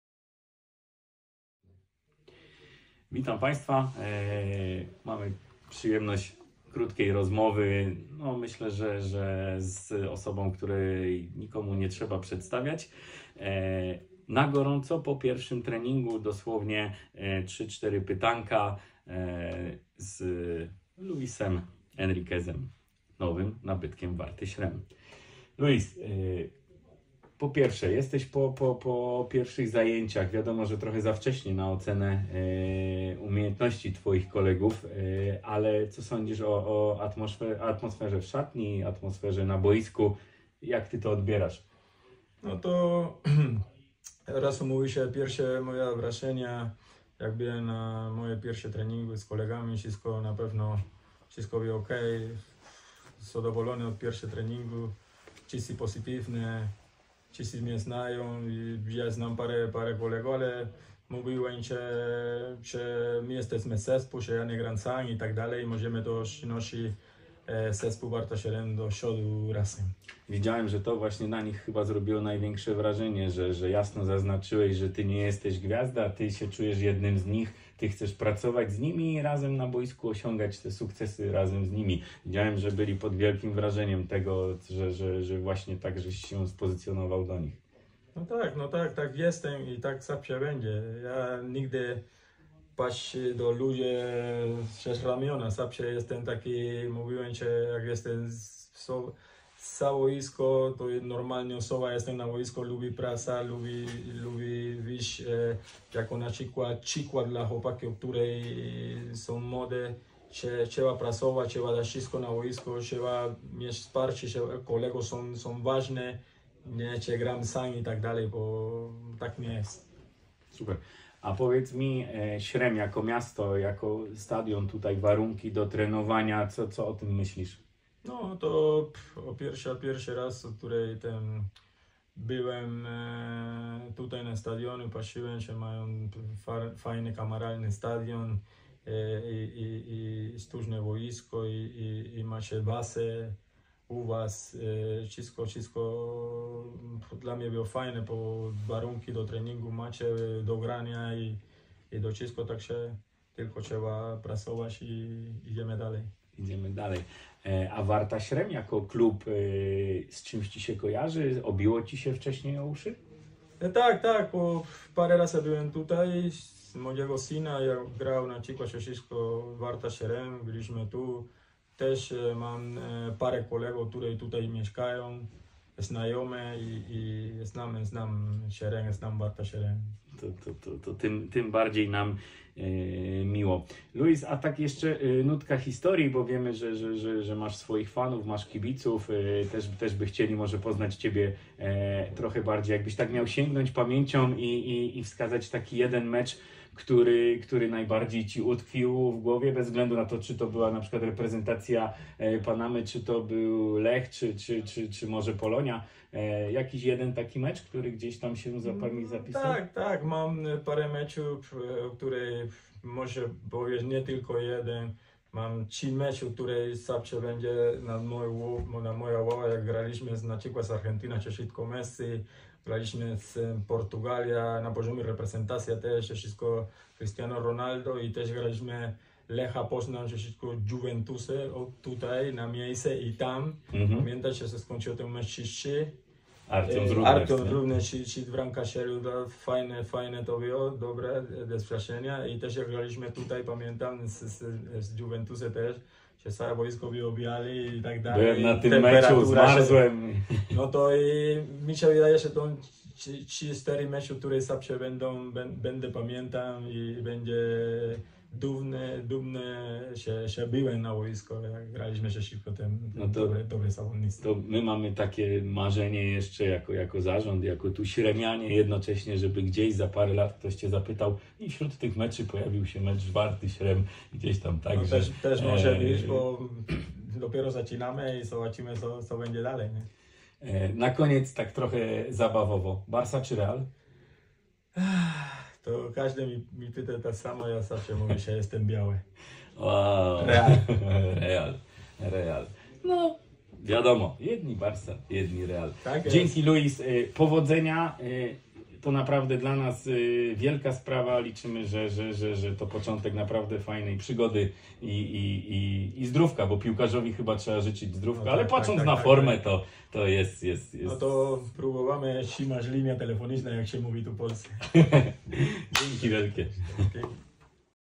Wywiad z Luisem Henriquezem, nowym zawodnikiem Śremskiego Klubu Sportowego Warta
Spontaniczny wywiad z Luisem, którego udzielił nam bezpośrednio po pierwszym treningu.